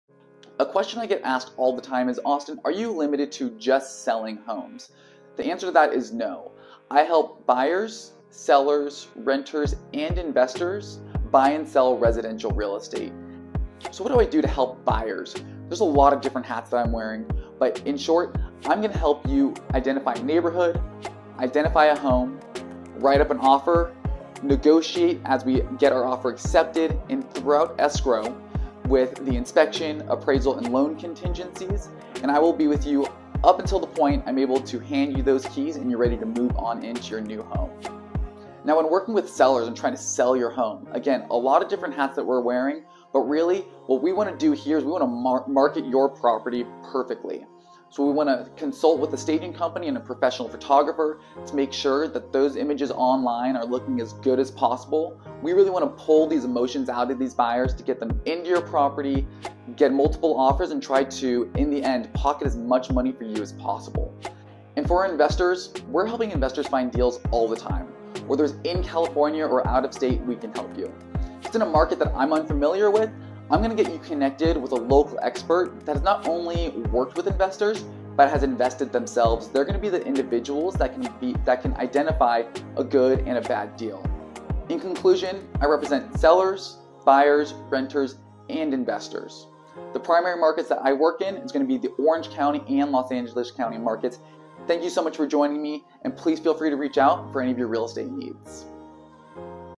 What is my role as a real estate agent?
As a real estate agent I help my clients buy, sell, rent, and invest in residential real estate throughout Orange County and Los Angeles County.

For buyers, I assist in connecting my clients with a mortgage lender to ensure they have the proper financing to purchase a home. Then I assist in narrowing down their search criteria to specific neighborhoods, I’ll schedule home tours, write up an offer to purchase the home, and negotiate on behalf of my buyer.

For sellers, I function as a marketing expert. My job is to identify the type of buyer that would purchase the home and market the home to that buyers wants and needs. My goal is to get as many eyes on the home as possible. The goal is to generate multiple offers and get the highest possible price for the sellers. Once an offer is accepted, my job is to negotiate on behalf of the seller to ensure that there is a smooth transaction and that the seller is able to pocket as much money as possible.

We help renters identify properties and write/ negotiate their rental agreements. For landlords, we would advertise the home as if we were going to sell it and help vet each applicant that applies to stay in the home.

Lastly, we help investors buy and sell real estate around the entire Unites States. If a property is in an area unknown to me, I can introduce you to an investor minded real estate agent that can best assist you.